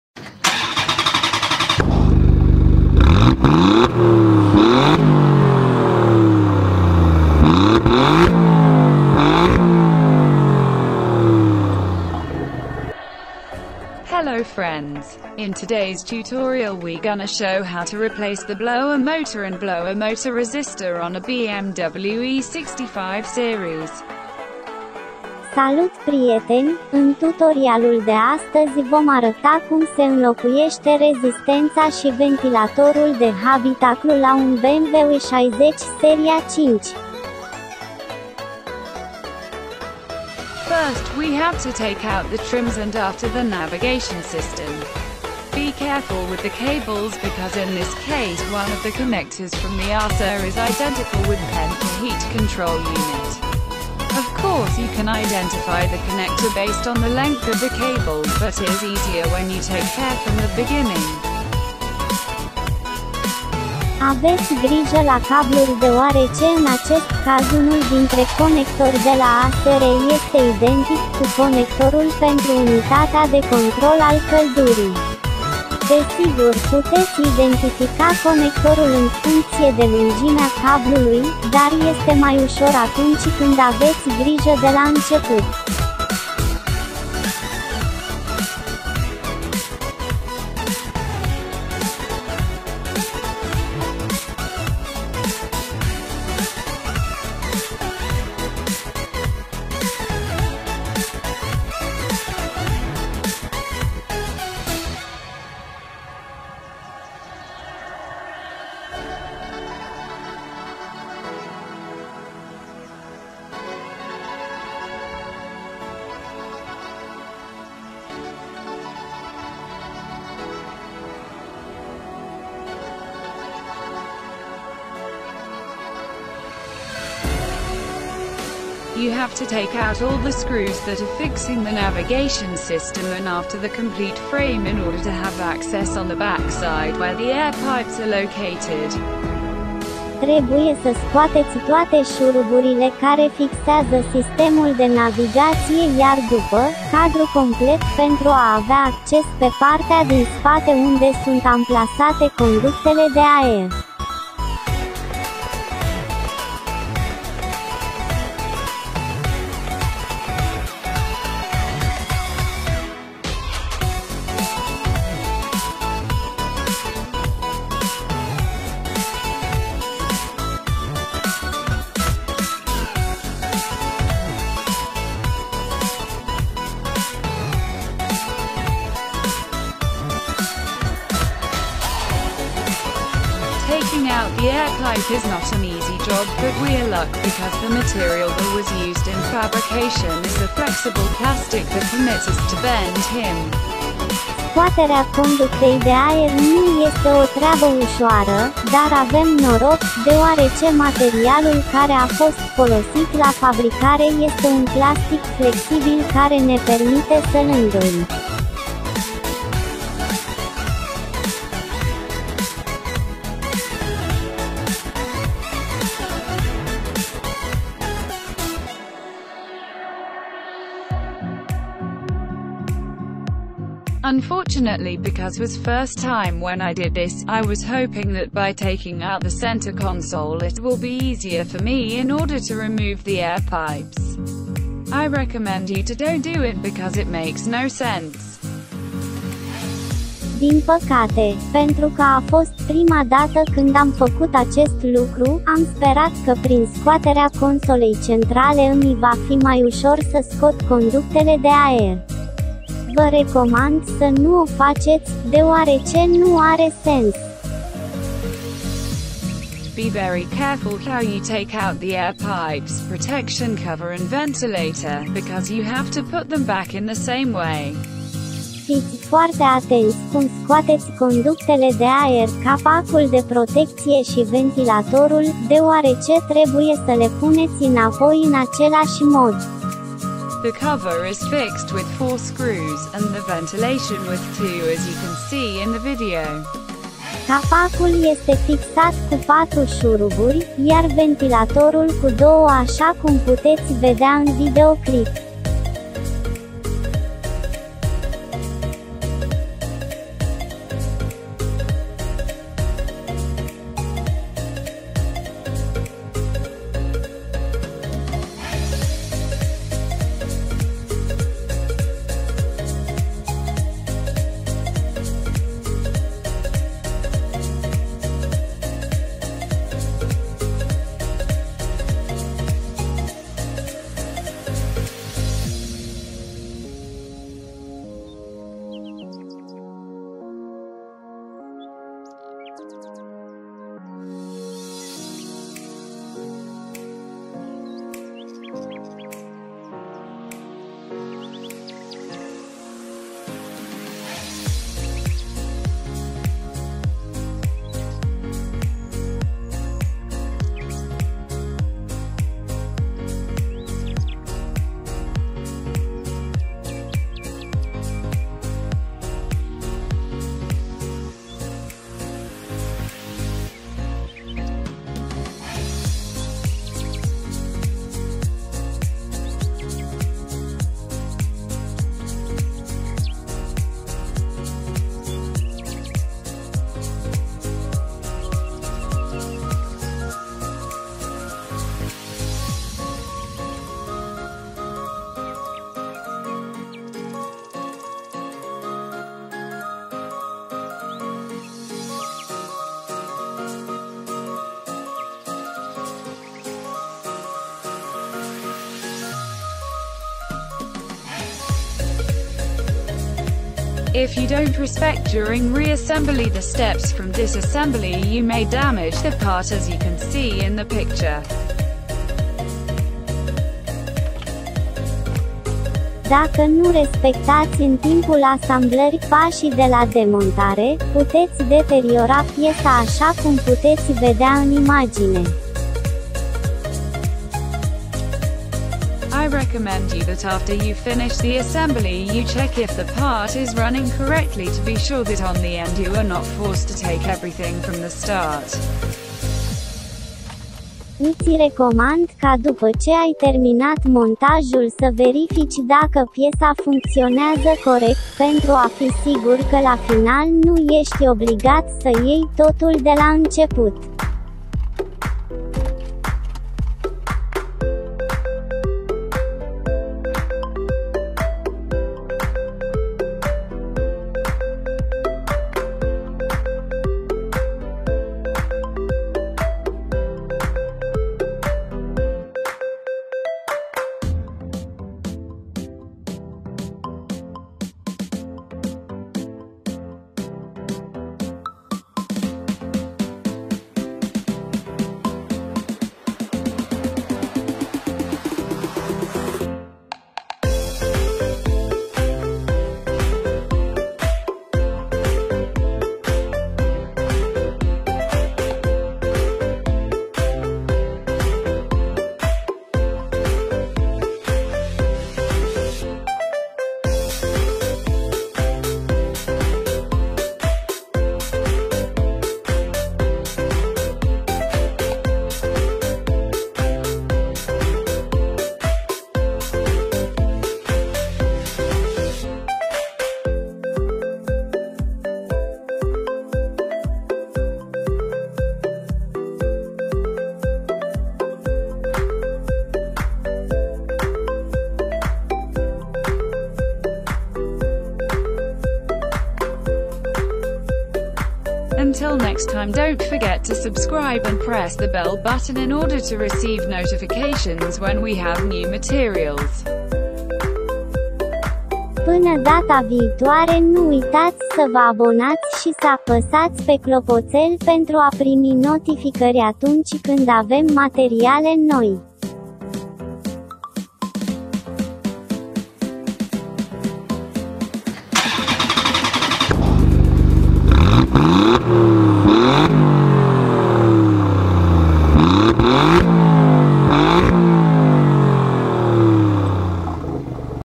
How to Replace the Blower Motor and Blower Motor Resister on a BMW E60 5 Series
How to Replace the Blower Motor and Blower Motor Resister on a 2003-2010 BMW E60 5 Series